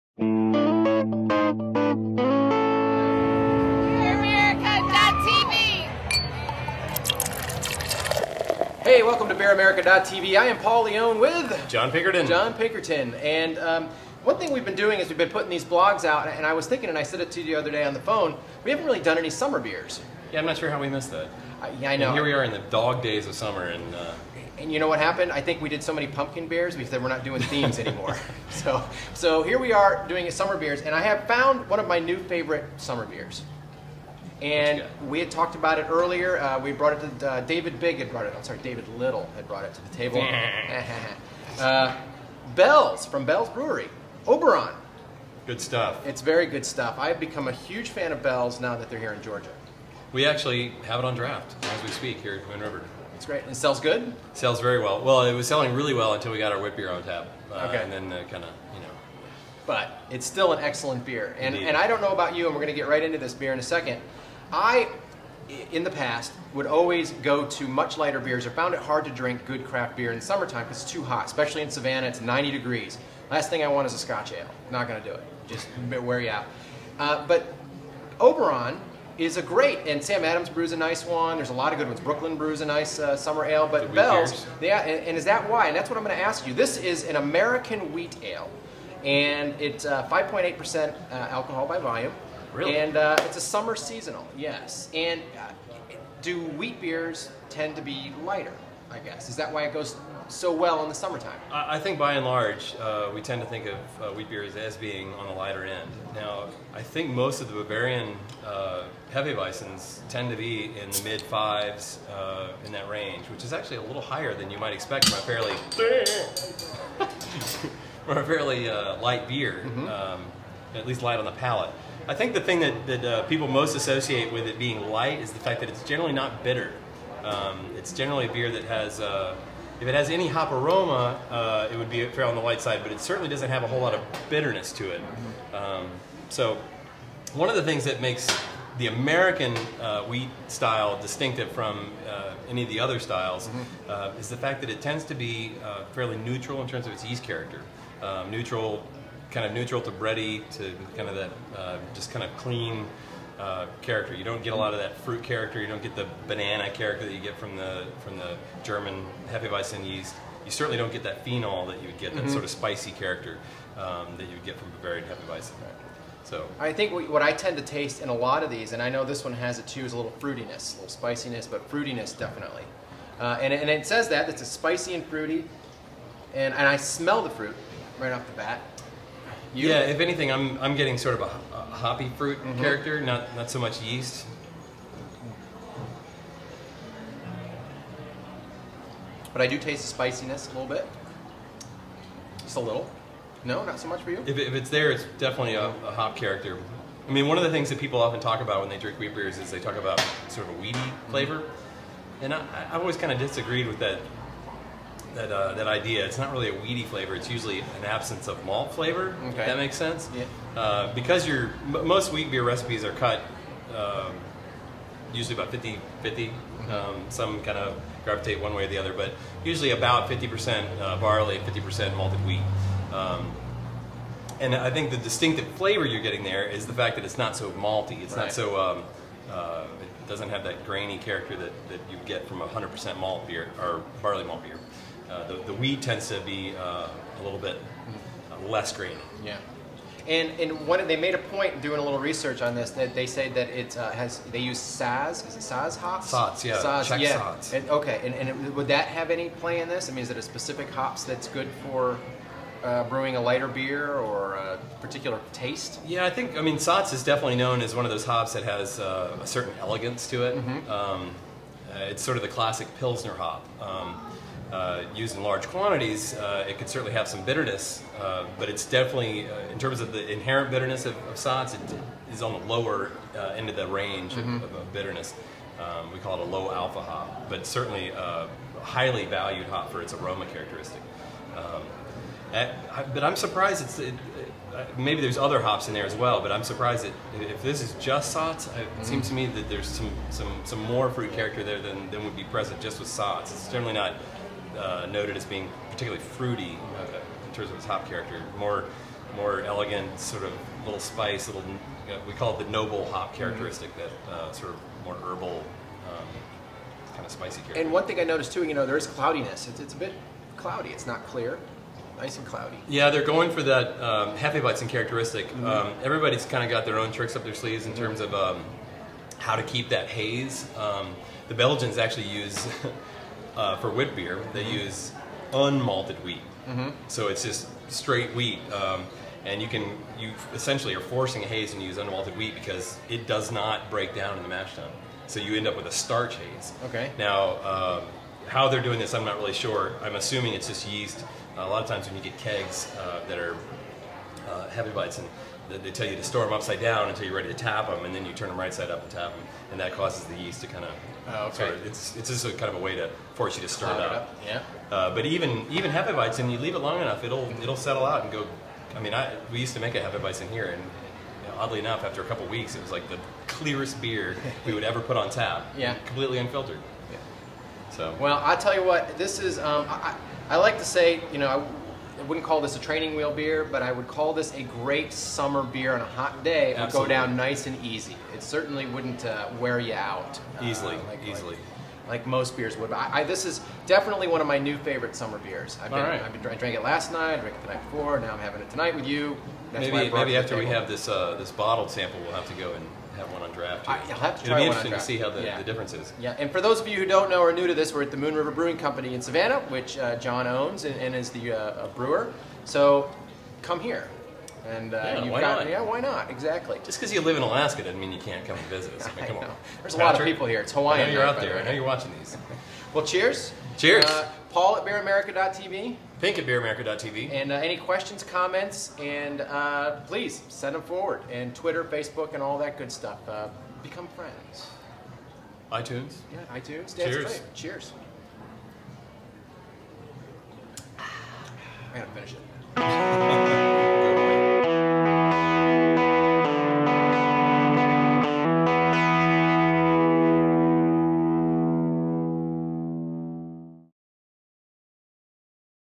Bell's Brewery, Oberon - Episode 73, Beer America TV
As the summer starts to wind down, we wanted to share an American Craft beer for a warm sunny day, Oberon. Oberon is an American wheat ale brewed with Saaz hops. It's spicy and fruity, but extremely well balanced. If you've not had beer from Bell's Brewery, we suggest you give them a try. This is our 5th Bell's beer that we've reviewed, and they've all been fantastic!  Distributed by Tubemogul.